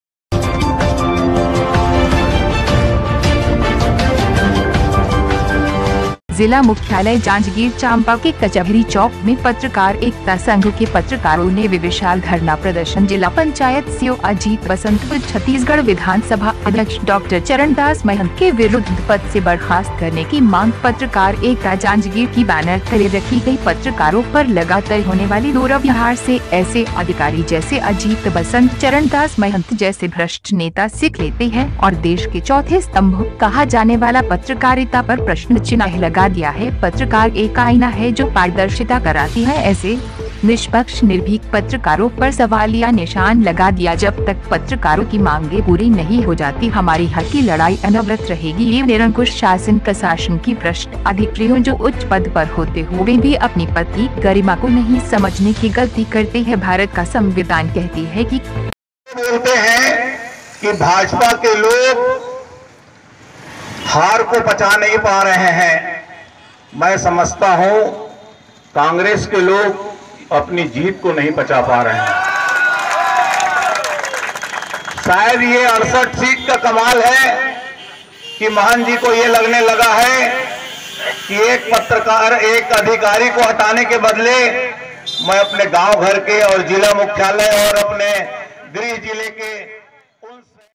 जांजगीर चाम्पा के कचहरी चौक मे पत्रकार एकता संघ के पत्रकारो ने विशाल धरना प्रदर्शन.जिलापंचायत सीओ
एंकर -जिला मुख्यालय जांजगीर चाम्पा के कचहरी चौक मे पत्रकार एकता संघ के पत्रकारो ने विशाल धरना प्रदर्शन.जिलापंचायत सीओ   अजीत बसंत व छतीसगढ़ विधान  सभा अध्यक्ष डा.चरण दास महंत के विरुध्द पद से बर्खास्त करने की मांग पत्रकार एकता जांजगीर की बैनर तले रखी गई.पत्रकारो पर लगातर होंने वाली दूरब्यहार से ऐसे अधिकारी जैसे अजीत बसंत.चरण दास महंत जैसे भ्रष्ट नेता सिख लेते है.और देश के चौथे स्तम्भ कहा जाने वाला पत्रकारिता पर प्रश्नचिन्ह लगा दिया है.पत्रकार एक आईना है जो पारदर्शिता कराती है ऐसे. निष्पक्ष निर्भीक पत्रकारो पर सवालिया निशान लगा दिया जब तक पत्रकारो की मांगे पूरी नही हो जाती हमारी हक की लड़ाई अनवरत रहेगी.यह निरंकुश शासन प्रसाशन की भ्रष्ट अधिकरियों जो उच्च पद पर होते हुवे भी अपनी पद की गरिमा को नही समझने की गलती करते है.भारत का संविधान कहती है की कानून सब के लिये बराबर है चाहे नाम चीन हो या सामन्य